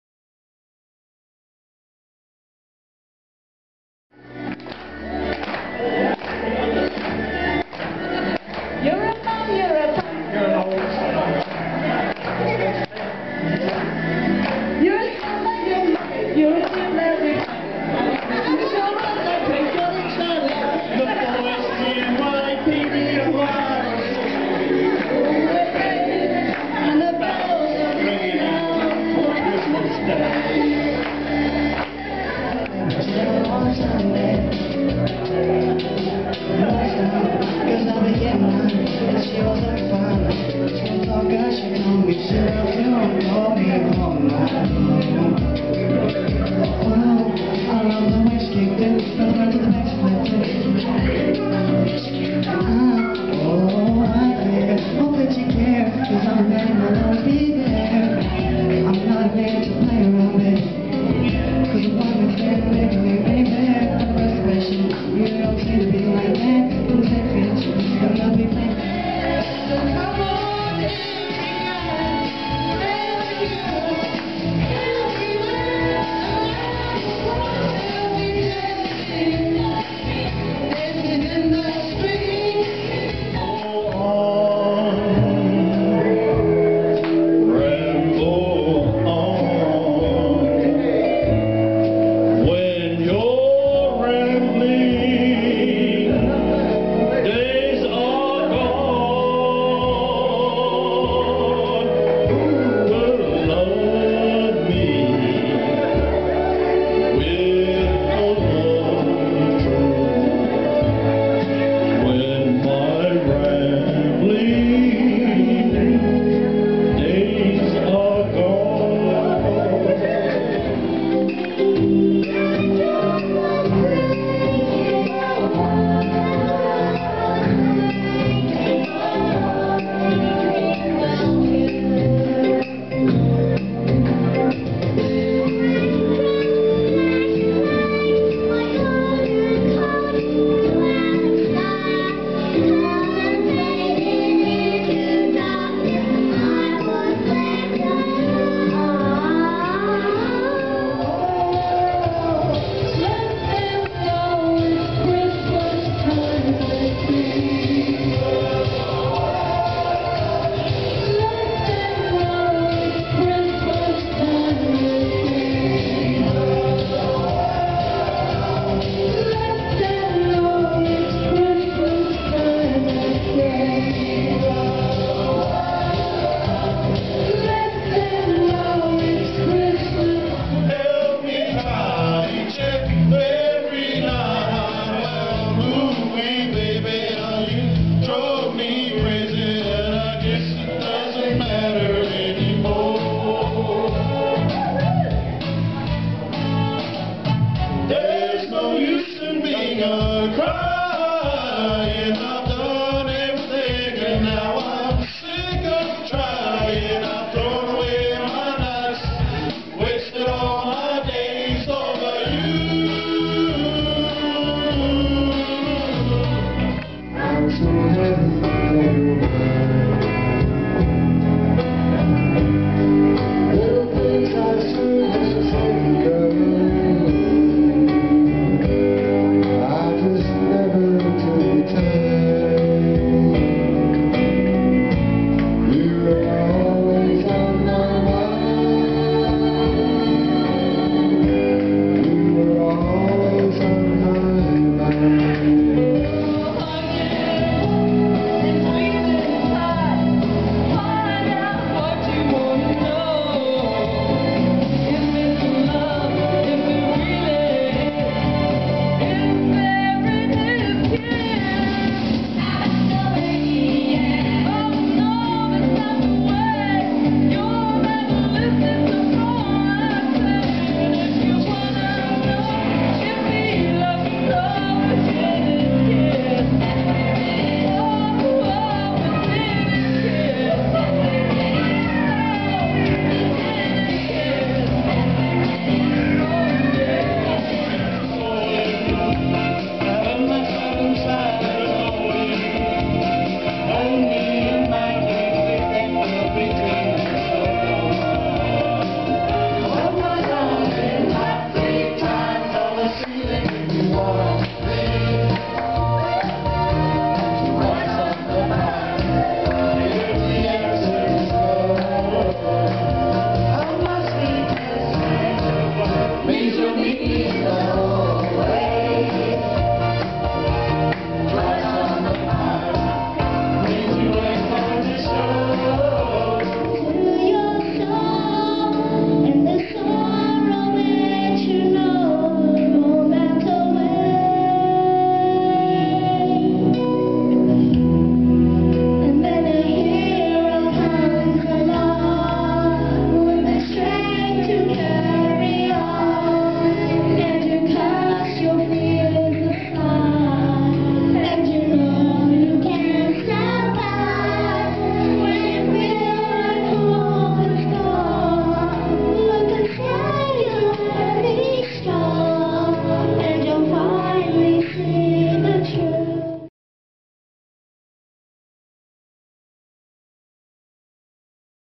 Nga Kakano Foundation Xmas Party 15 12 07
A Variety of video clips from an excellent Christmas Party!  Enjoy!